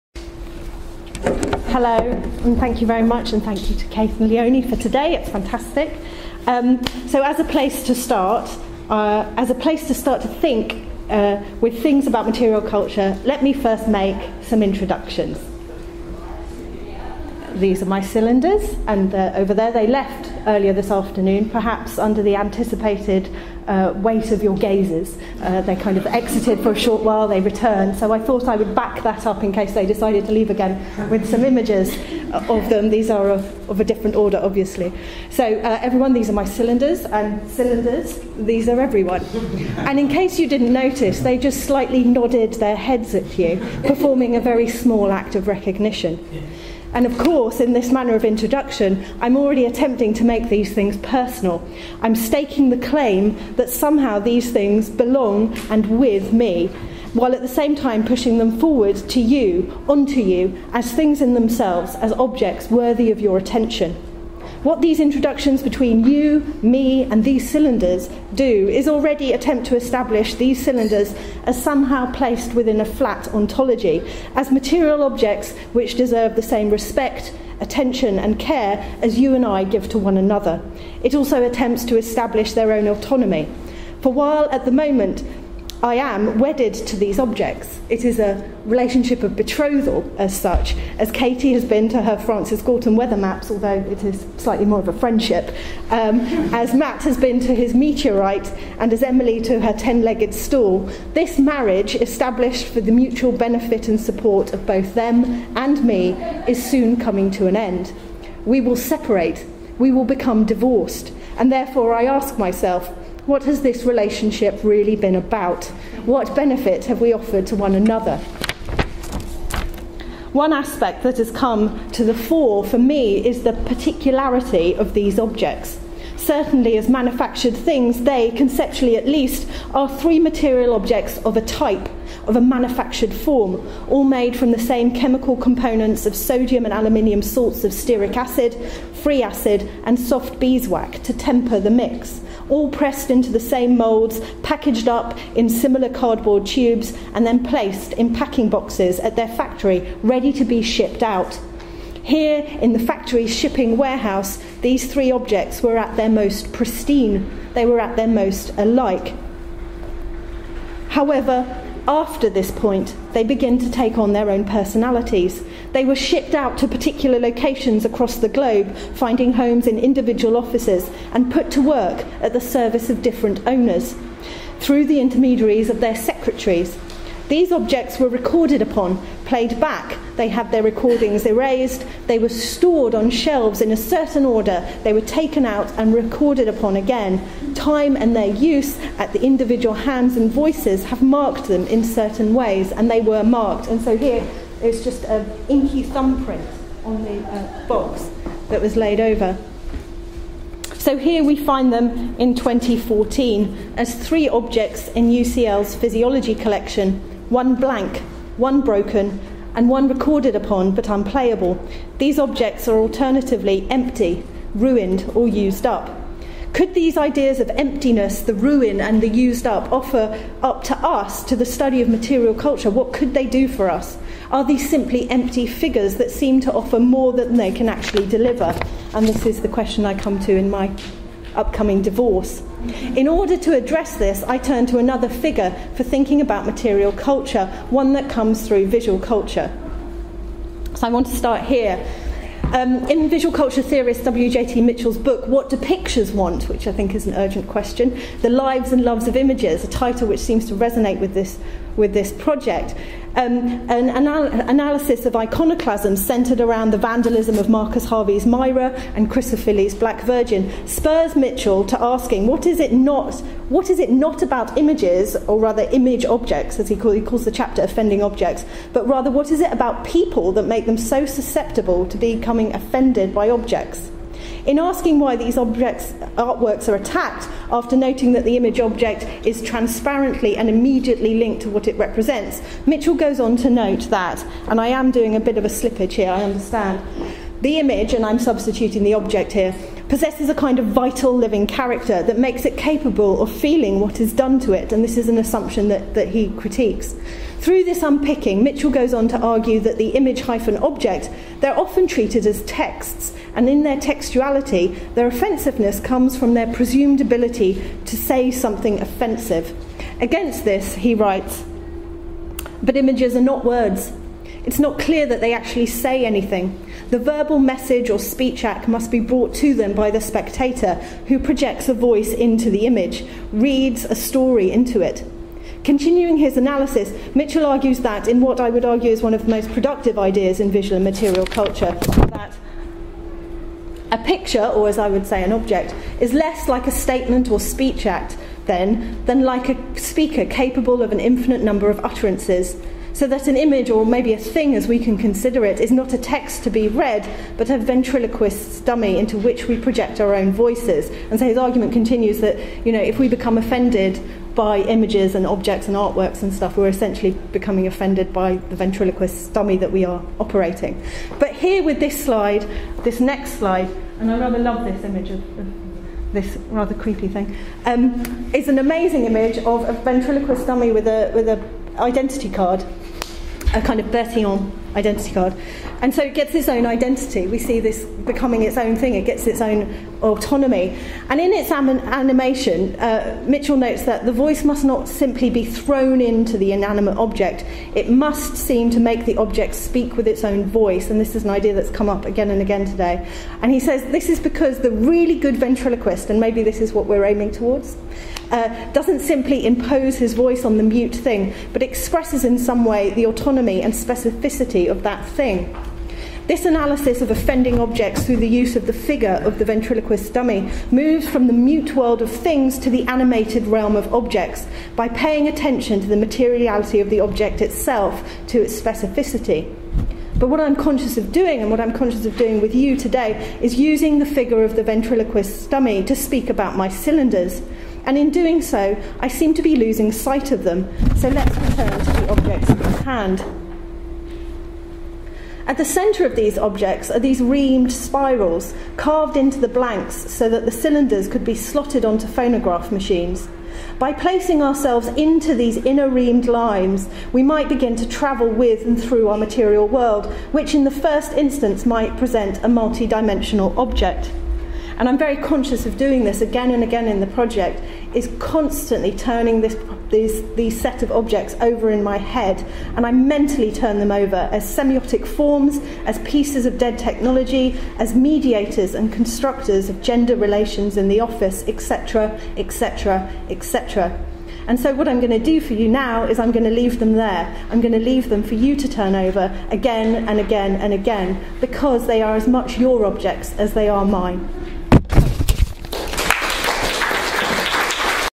Juliette Kristensen
Juliette Kristensen speaking on 'The Object and ... and ... and ... and ...' at the 100 Hours Forum at University College London, June 2014.
[Edison Dictaphone Tubes, Physiology Collections, UCL]